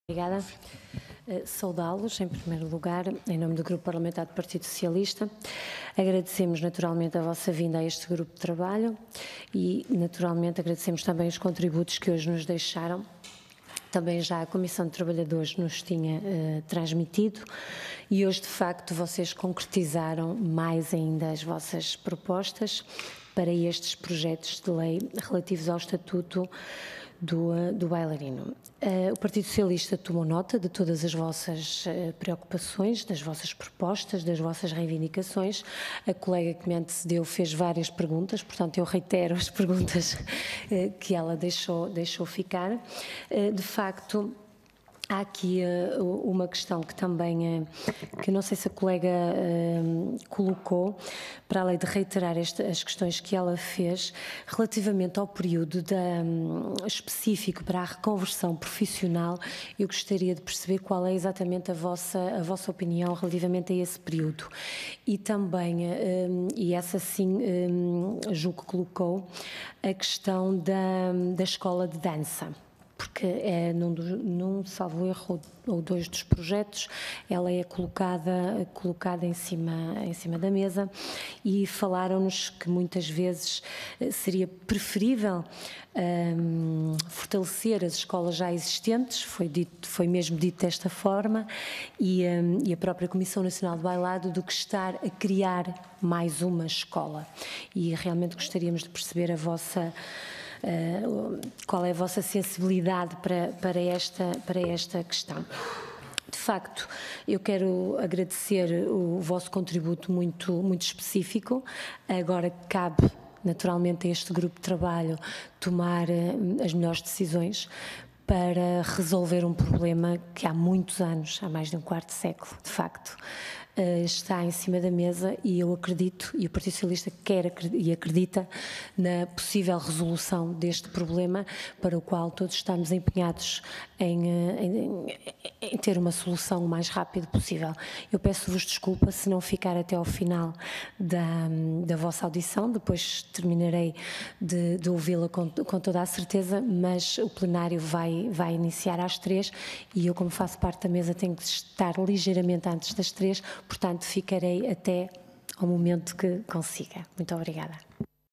02-05-2018 | Audição do CENA - STE | Sandra Pontedeira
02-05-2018 | Intervenção da Deputada Sandra Pontedeira - Audição do CENA - STE (Sindicato dos Trabalhadores de Espetáculos, do Audiovisual e dos Músicos) - Comissão de Trabalho e Segurança Social